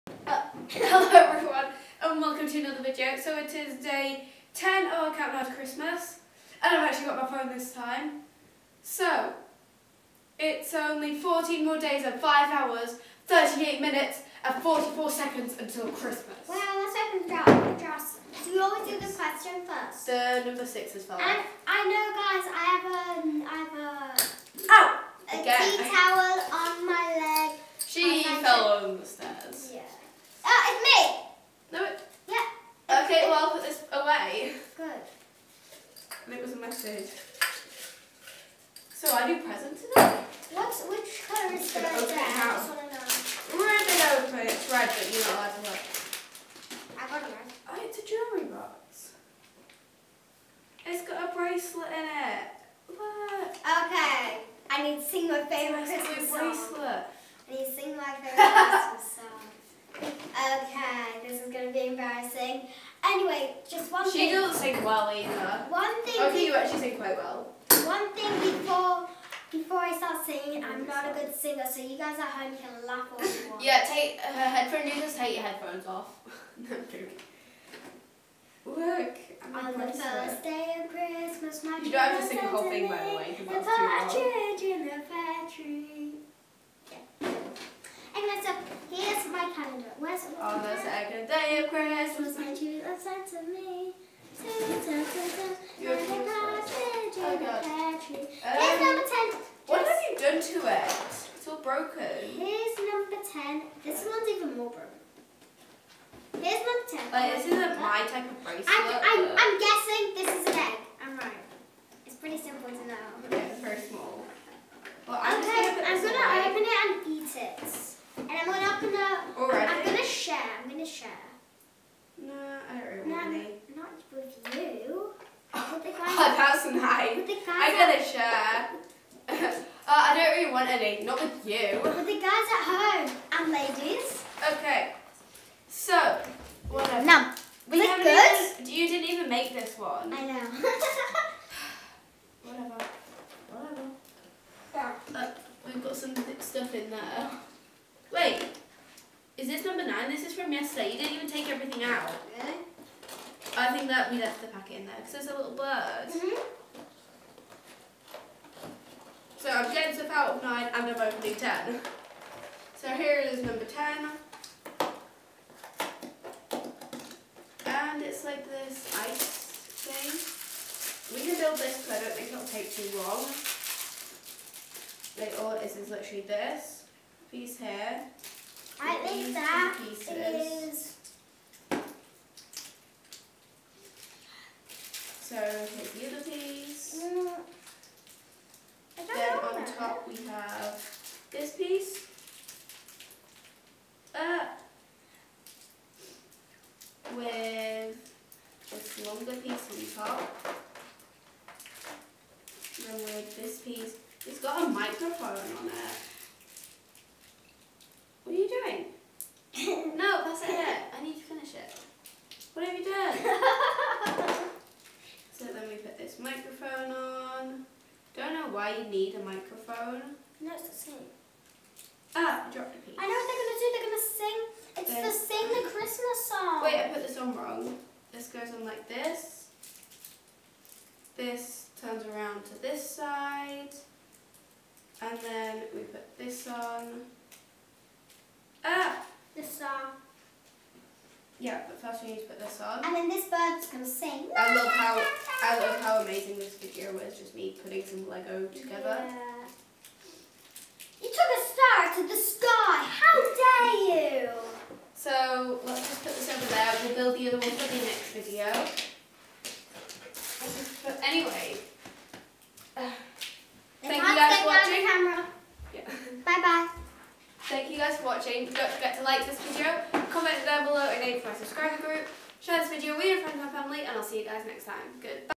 COUNT DOWN TO CHRISTMAS: ep 10
In this video it is day 10 of the count down to christmas with my little sister ad its where me and her count down the days until Christmas with you guys.